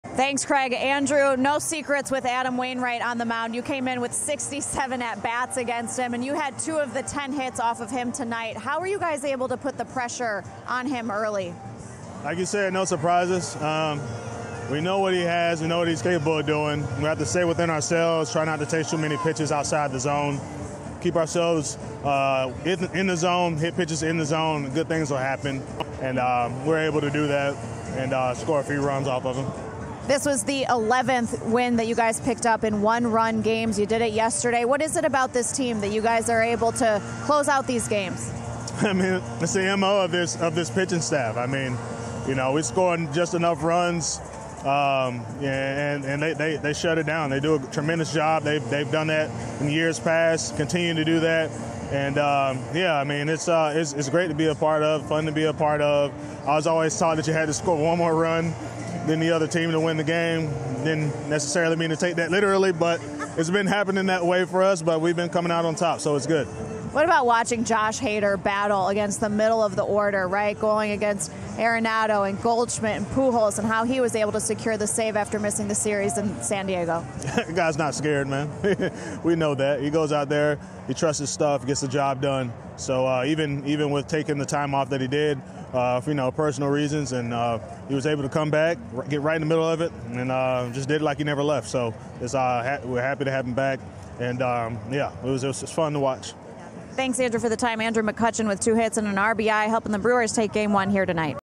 Brewers' McCutchen on Hader: 'That guy's not scared'
Andrew McCutchen talks about Josh Hader stepping right back into a big moment and the Brewers grabbing another close win.